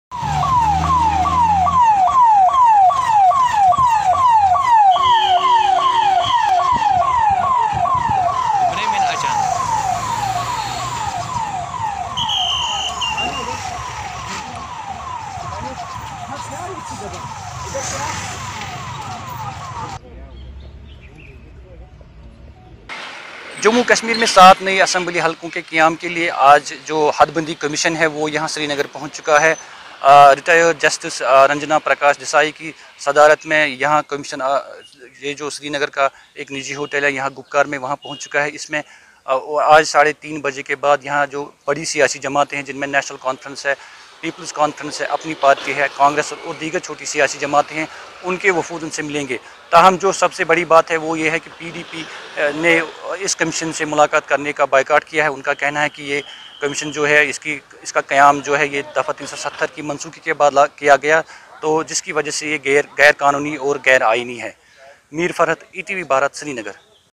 Delimitation commission in jammu and kashmir
چوبیس جون کو دہلی میں مختلف مین اسٹریم سیاسی رہنماؤں کے ساتھ وزیر اعظم نریندر مودی کی ملاقات کے بعد حد بندی کو حتمی شکل دینے کے احکامات صادر کیے گئے حالانکہ کشمیر کی کئی سیاسی جماعتوں نے حدبندی کے بارے میں اپنے تحفظات کا اظہار کیا ہے۔